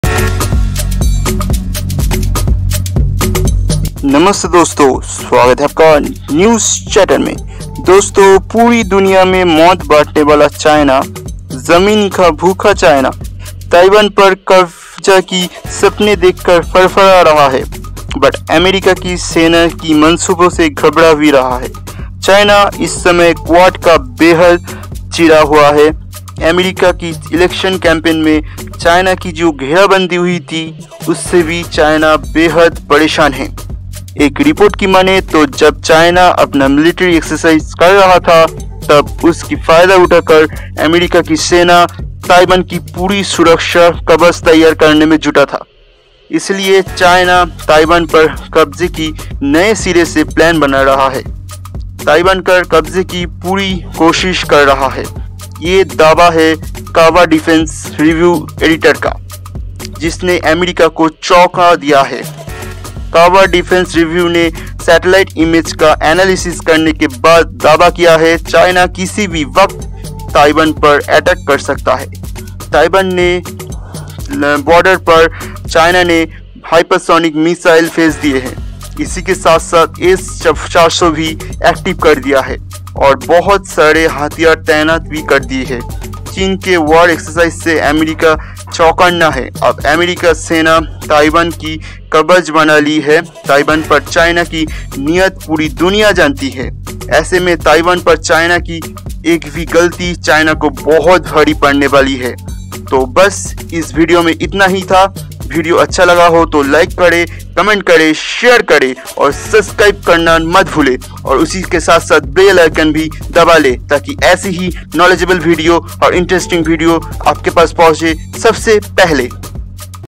American Army Deployed on Taiwan.USA सेना का खौफ चीन पर
American Army Deployed On Taiwan.ताईवान पर तैनात अमेरिकी सेना.

चीन पर usa का डर.

News Chatter

mix